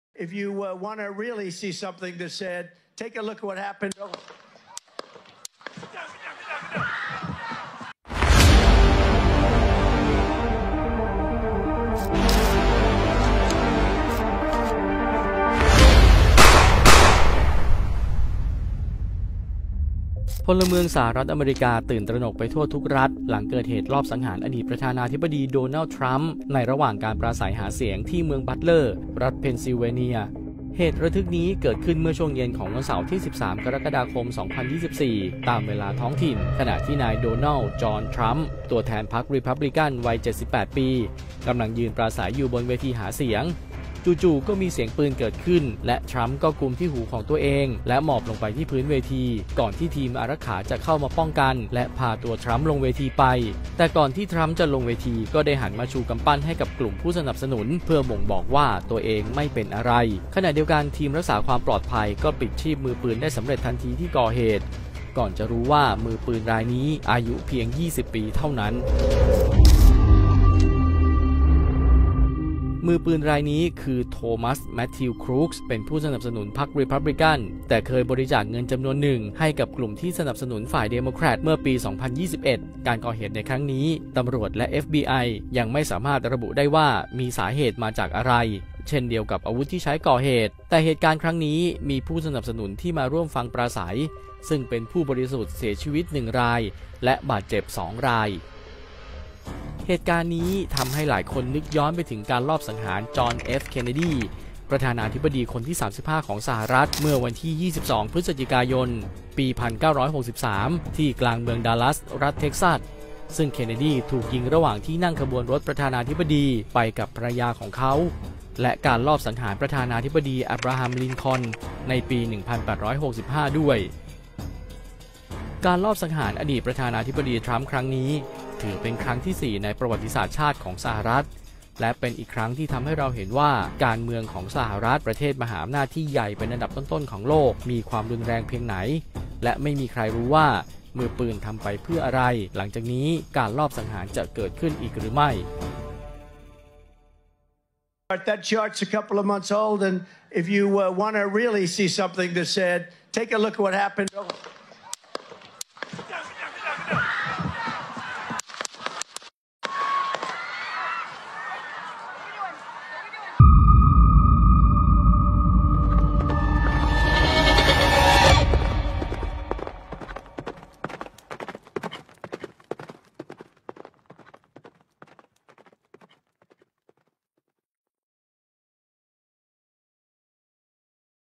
โดนัลด์ ทรัมป์ ถูกลอบยิงกลางเวทีหาเสียง | อีจัน EJAN
วินาทีช็อกโลก โดนัลด์ ทรัมป์ อดีตประธานาธิบดีสหรัฐฯ ถูกลอบยิงกลางเวทีหาเสียง ตำรวจเร่งสาวปมลั่นไก